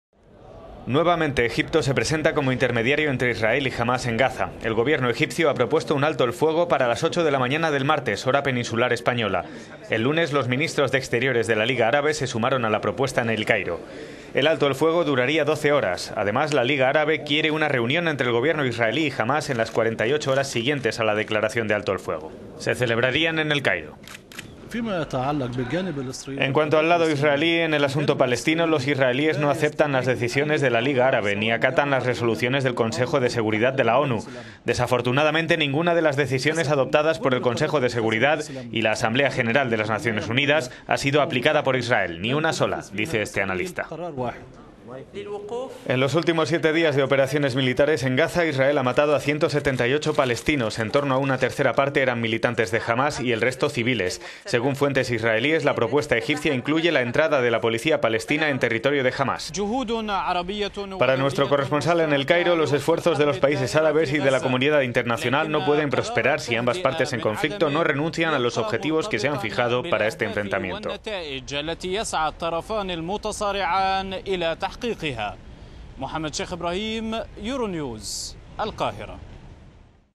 La Liga Árabe respalda la propuesta egipcia de alto el fuego en Gaza
Egipto ha presentado la primera propuesta de alto el fuego para los enfrentamientos entre Israel y Hamás en Gaza. El régimen de Al Sisi retoma el papel de Egipto como intermediario entre ambas partes y propone un alto el fuego desde las 8 de la mañana, hora peninsular española. Los ministros de exteriores de la Liga Árabe se han sumado la propuesta en una reunión extraordinaria celebrada en El Cairo.

De ser aceptado, el alto el fuego duraría 12 horas. Además, la Liga Árabe propone que se celeb...